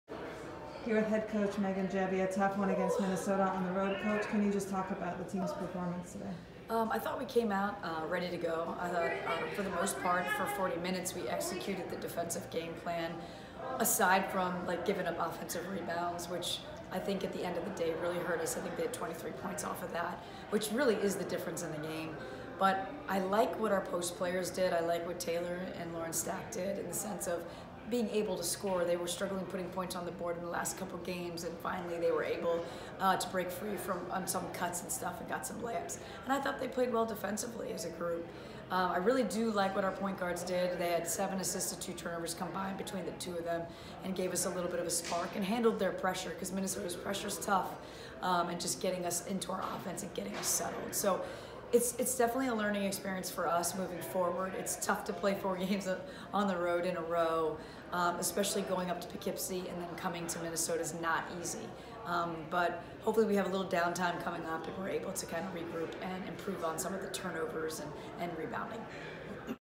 AU Women's Basketball Postgame vs. Minnesota - Head Coach Megan Gebbia
Head Coach Megan Gebbia recaps AU's 73-56 road loss to Minnesota on Wednesday, Nov. 17, 2021.